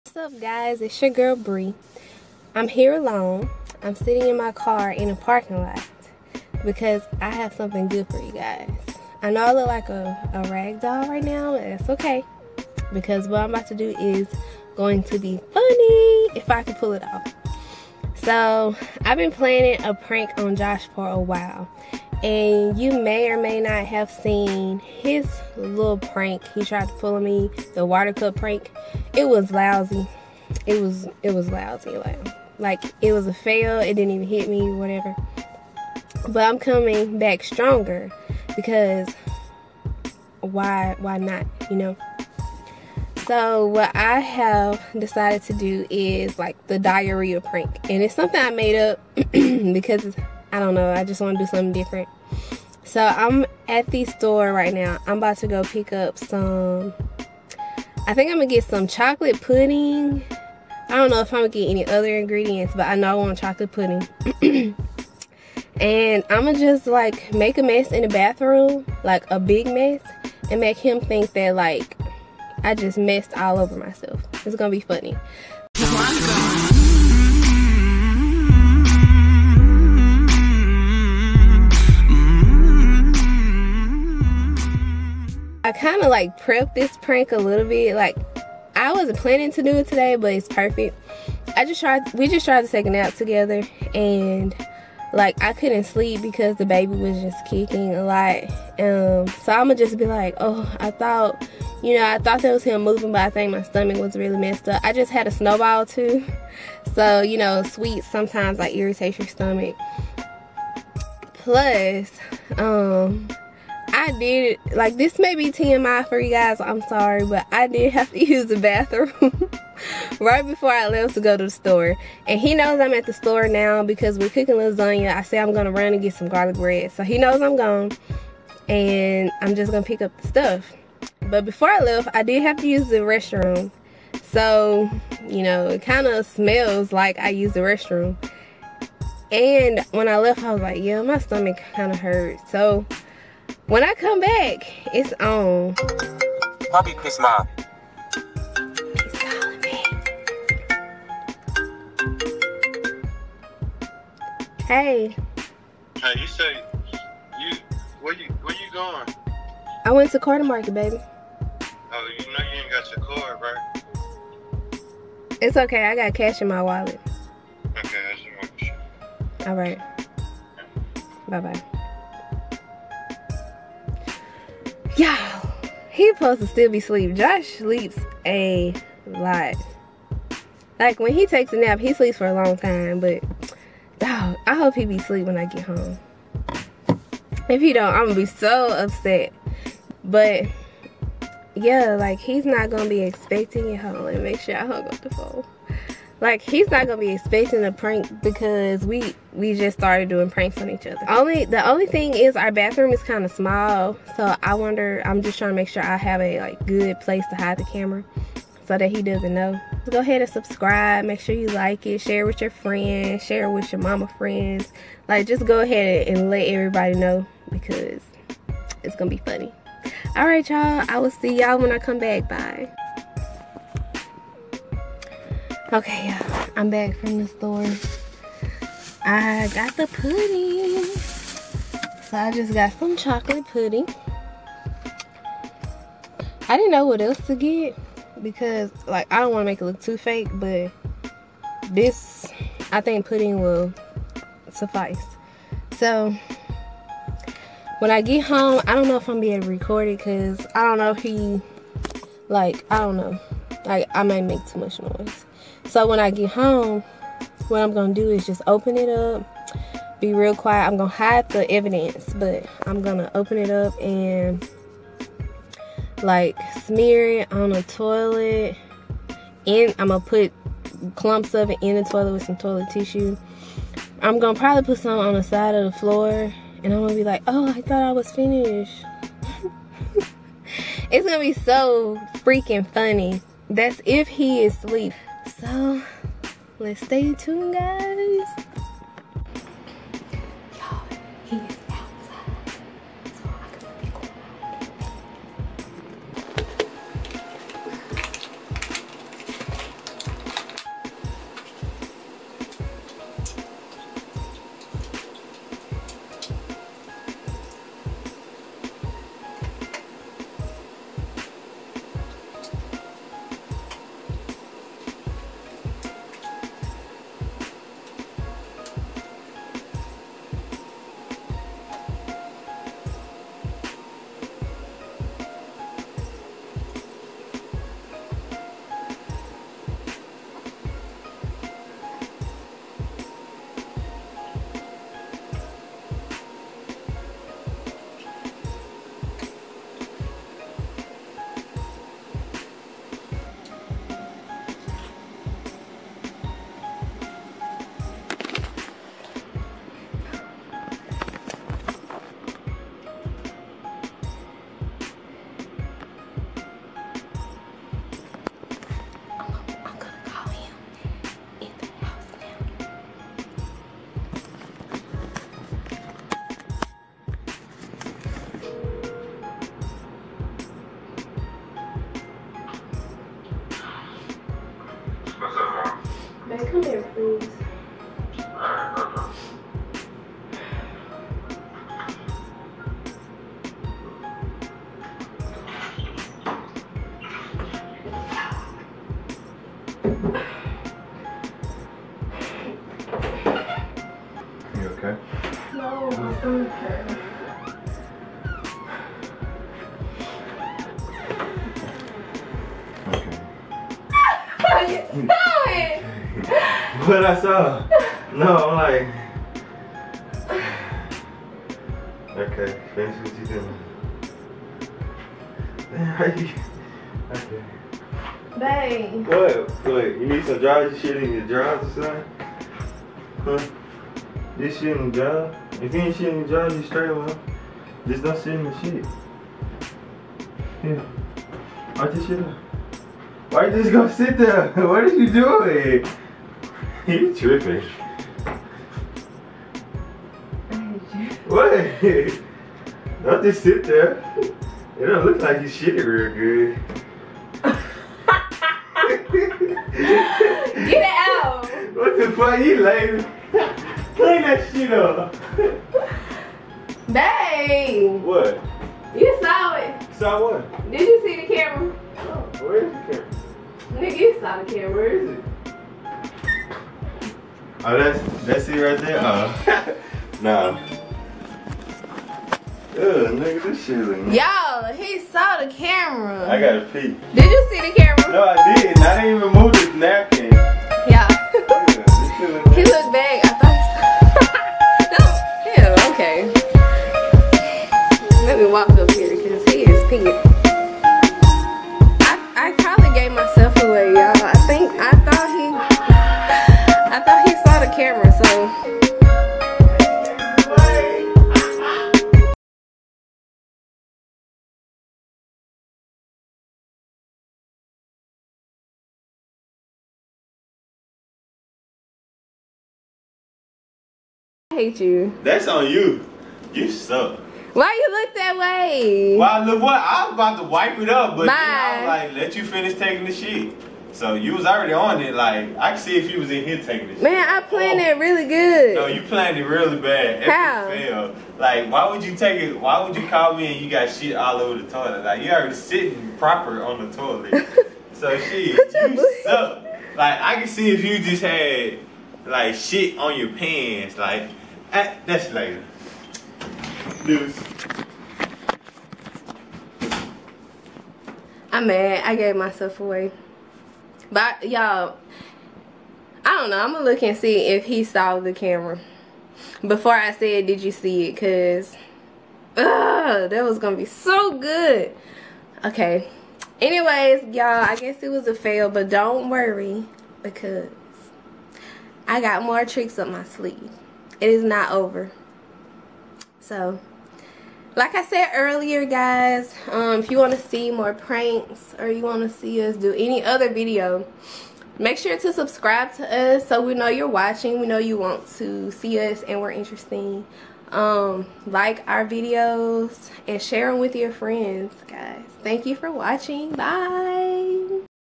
Diarrhea Prank! Fail
I thought I got him good with this one, but I gave myself away!

Sorry about the video glitch, the editor failed me :(